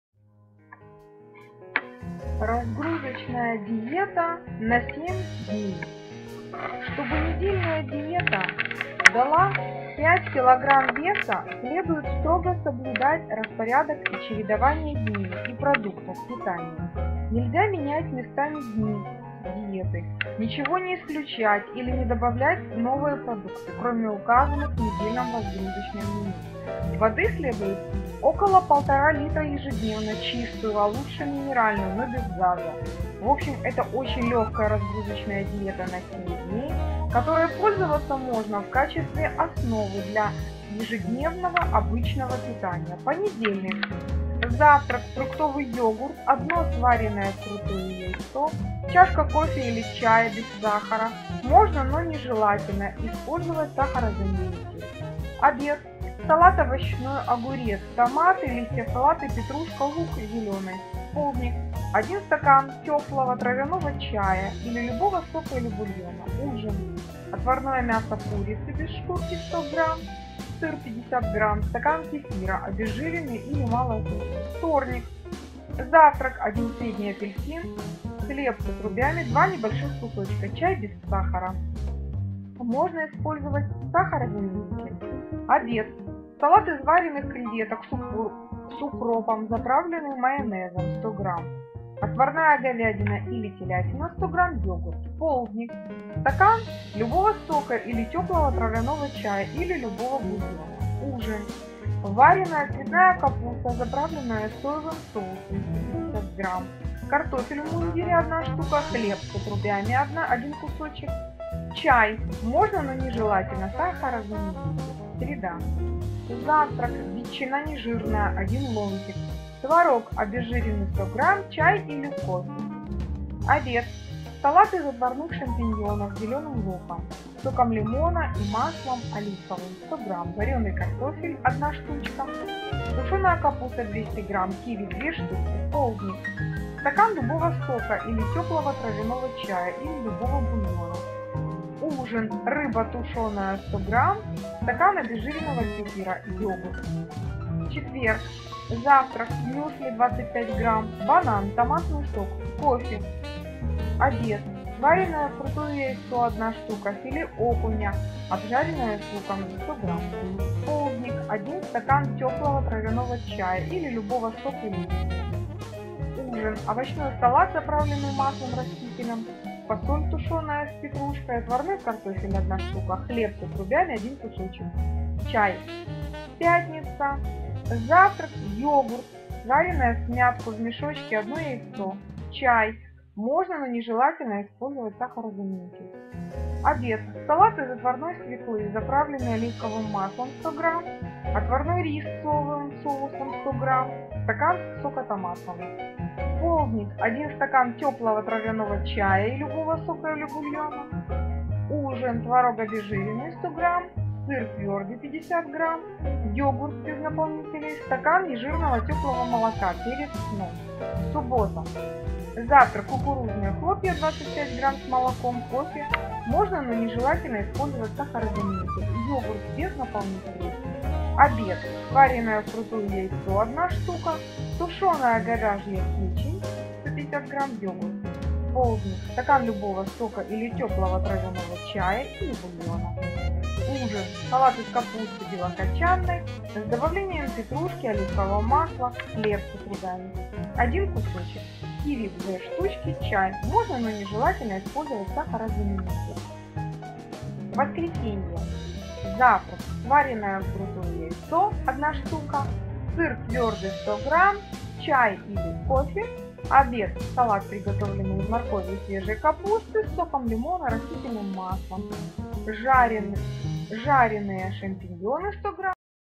КАК ПОХУДЕТЬ НА 10 КГ ЗА НЕДЕЛЮ? Разгрузочная неделя на 7 дней
РАЗГРУЗОЧНАЯ ДИЕТА НА 7 ДНЕЙ
 • ПОНЕДЕЛЬНИК 1. Завтрак: фруктовый йогурт, одно сваренное вкрутую яйцо, чашка кофе или чая без сахара
2. Обед: салат овощной (огурец, томаты, листья салата, петрушка, лук зел3. Полдник: один стакан теплого травяного чая или любого сока или бульона. 4. Ужин: отварное мясо курицы без шкурки - 100 гр., сыр - 50 гр., стакан кефира (обезжиренный или маложирный) • ВТОРНИК 1. Завтрак: один средний апельсин, хлеб с отрубями - 2 небольших кусочка, чай без сахара 2. Обед: салат из вареных креветок с укропом, заправленный майонезом - 100 гр., отварная говядина или телятина - 100 гр., йогурт. 3. Полдник: стакан любого сока или теплого травяного чая или любого бульона
4. Ужин: вареная цветная капуста, заправленная соевым соусом - 150 гр., картофель в мундире – 1 шт., хлеб с отрубями – 1 кусочек, чай
• СРЕДА 1. Завтрак: ветчина нежирная – 1 ломтик, творог обезжиренный - 100 гр., чай или кофе 2. Обед: салат из отварных шампиньонов с зеленым луком, соком лимона и маслом оливковым - 100 гр., вареный картофель – 1 шт., тушеная капуста - 200 гр., киви – 2 шт. 3. Полдник: стакан любого сока или теплого травяного чая или любого бульона
 4. Ужин: рыба тушеная - 100 гр., стакан обезжиренного кефира, йогурт без наполнителей
. • ЧЕТВЕРГ 1. Завтрак: мюсли - 25 гр., банан, томатный сок, кофе2. Обед: сваренное вкрутую яйцо – 1 шт., филе окуня, обжаренное с луком - 100 гр. 3. Полдник: один стакан теплого травяного чая или любого сока или бульона. 4. Ужин: овощной салат, заправленный маслом растительным, фасоль тушеная с петрушкой, отварной картофель - 1 шт., хлеб с отрубями – 1 кусочек, чай • ПЯТНИЦА 1. Завтрак: йогурт, сваренное всмятку («в мешочек») яйцо – 1 шт., чай
 2. Обед: салат из отварной свеклы, заправленный оливковым маслом - 100 гр., отварной рис с соевым соусом - 100 гр., стакан сока томатного. 3. Полдник: один стакан теплого травяного чая или любого сока или бульона.
 4. Ужин: творог обезжиренный - 100 гр., сыр твердый - 50 гр., йогурт без наполнителей, стакан нежирного теплого молока перед сном.
 • СУББОТА 1. Завтрак: кукурузные хлопья (25 гр.) с молоком, кофе2. Обед: сваренное вкрутую яйцо – 1 шт., тушеная говяжья печень - 150 гр., йогурт. 3. Полдник: стакан любого сока или теплого травяного чая или любого бульона
4. Ужин: салат из капусты краснокочанной, с добавлением петрушки и оливкового масла, хлеб с отрубями – 1 кусочек, киви – 2 шт., чай ВОСКРЕСЕНЬЕ1. Завтрак: сваренное вкрутую яйцо – 1 шт., сыр твердый - 100 гр., чай или кофе 2. Обед: салат, приготовленный из моркови, и свежей капусты, с соком лимона и маслом растительным, жареные шампиньоны - 100 гр., отварной рис - 100 гр., сок апельсиновый – 1 стакан
3Полдник: один стакан теплого травяного чая или любого сока или бульона. 4. Ужин: крабовые палочки, политые лимоном - 50 гр., тушенная говяжья печень - 150 гр., стакан теплого молока перед сном